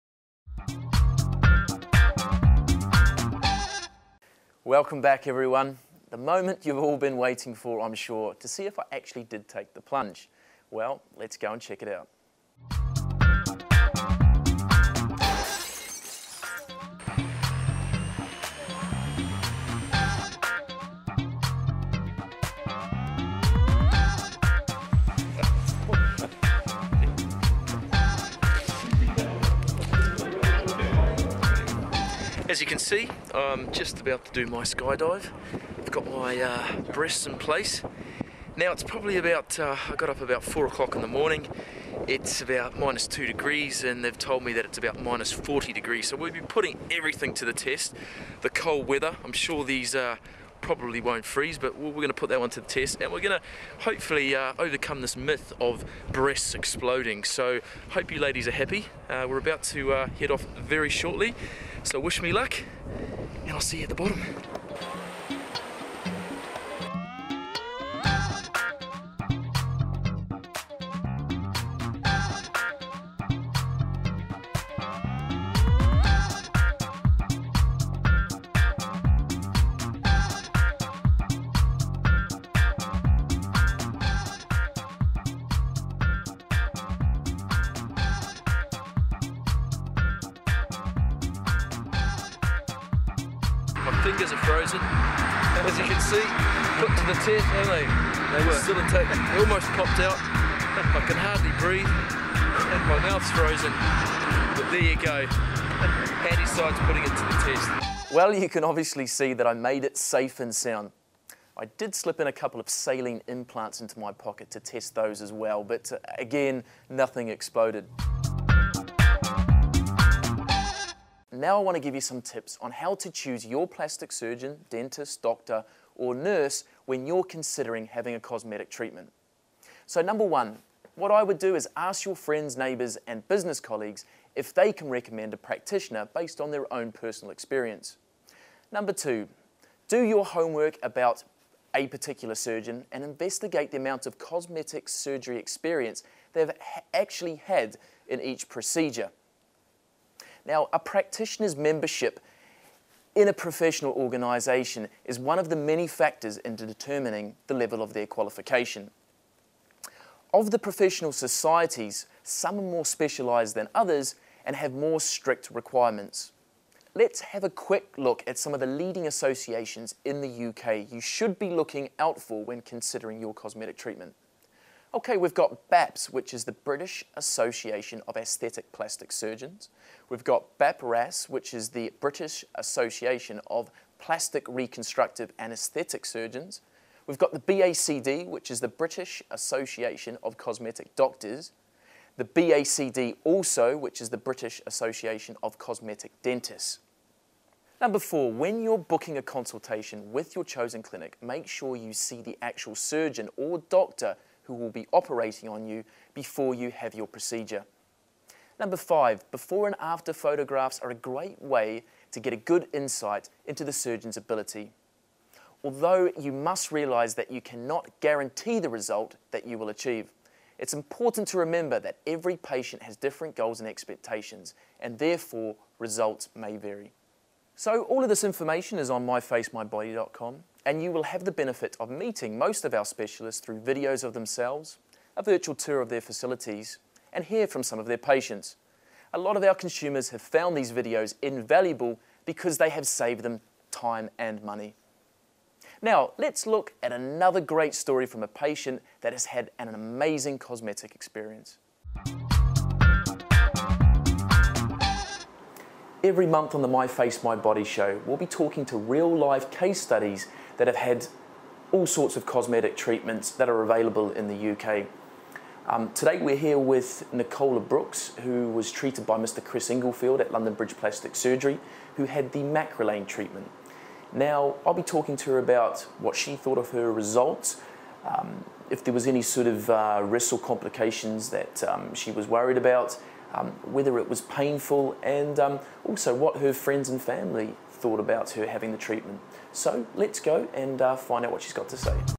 Do Breast Implants Explode when in an Aeroplane?
Do breast Implants REALLY Burst in High Altitudes? Surely not in a plane? Watch Stephen Handisides jump out of a plane to bust the myth around exploding breasts.  Find out what happens?